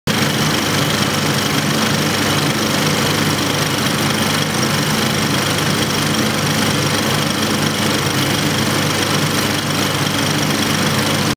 Old Movie Projector in Action.
I work at a movie theater and took this footage back in 2011, when we still had some of these old-fashioned projectors (by now, they're all digital).

What you basicallly see is how the film-strip is being pulled through the projector. Though it seems like a smooth motion, the film strip is actually being stopped and then moved again 24 times per second!